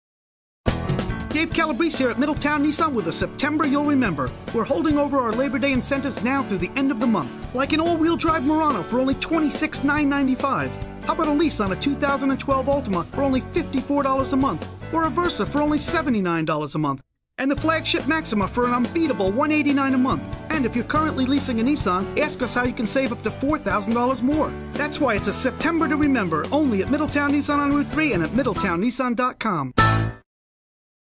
Middletown Nissan - Remember Sept 2011
It's a September you'll remember at Middletown Nissan. Labor Day Incentives have been extended through the end of September and that means great deals on a new Nissan.